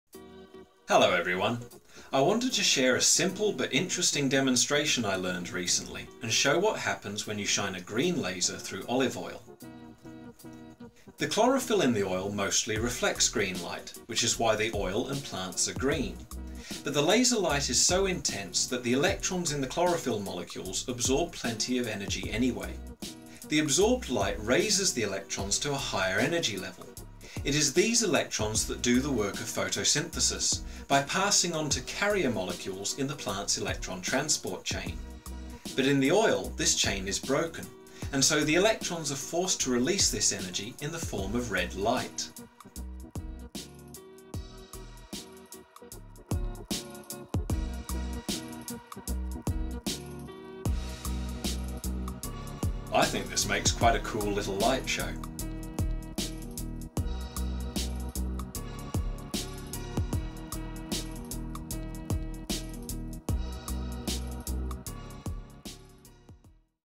Lasers and Olive Oil
When you shine a green laser through olive oil the oil fluoresces red. The chlorophyll in the oil absorbs the light, but because the plant cells have been broken open, the photosynthesis chain is broken and the electrons from the chlorophyll molecules release the energy as red light, making an interesting little light show.